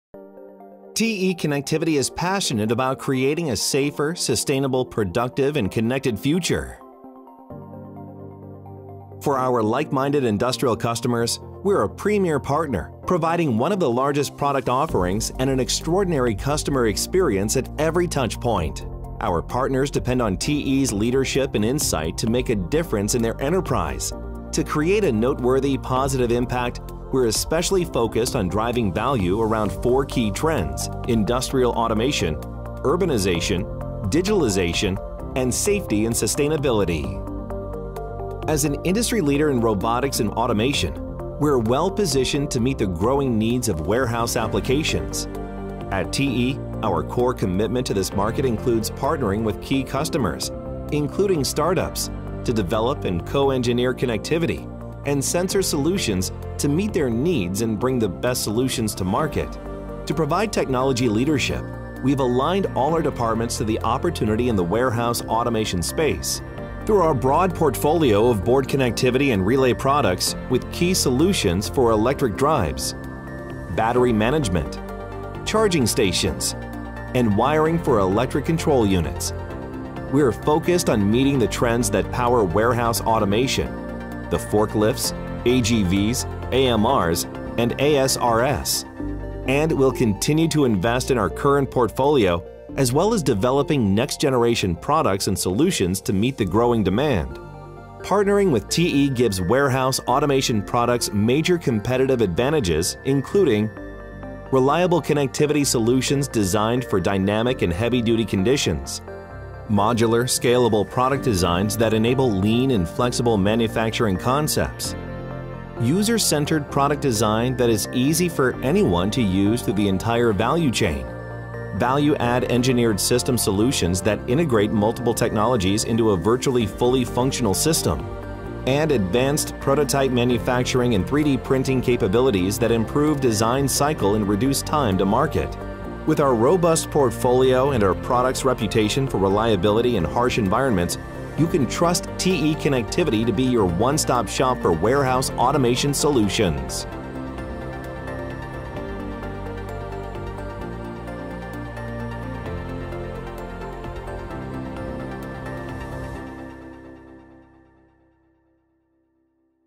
Optimized Warehouse Automation with TE Connectivity | Heilind Electronics
TE Connectivity is committed to developing connectivity and sensor solutions designed to meet the needs of an expanding warehouse automation market.